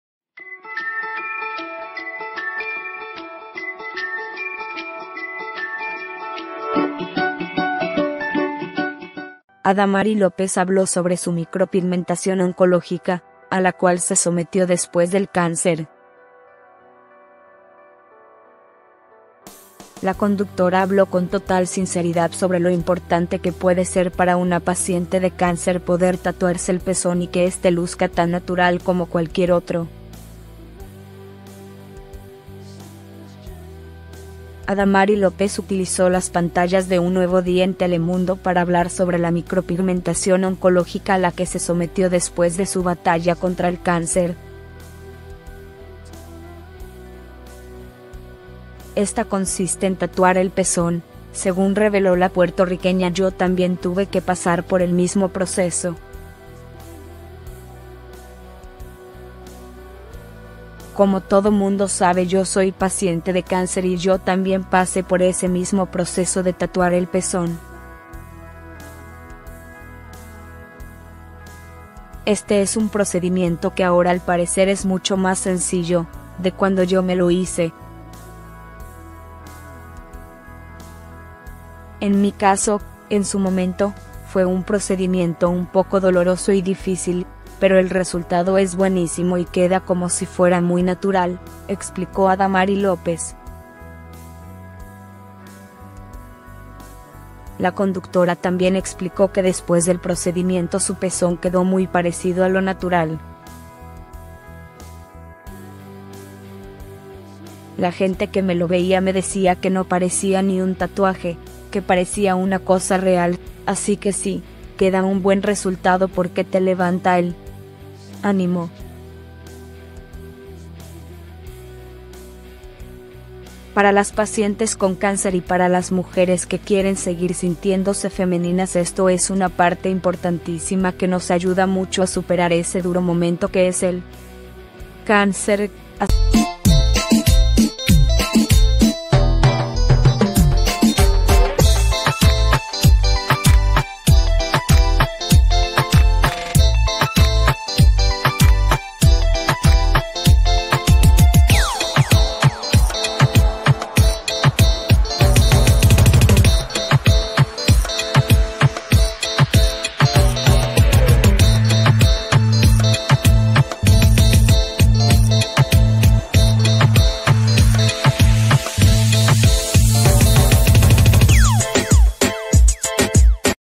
Adamari López se derrumbó cuando el médico anunció: ""Ella tiene solo 2 semanas para vivir"
Adamari López se derrumbó cuando el médico anunció: ""Ella tiene solo 2 semanas para vivir" AdamariLópez 247Noticias.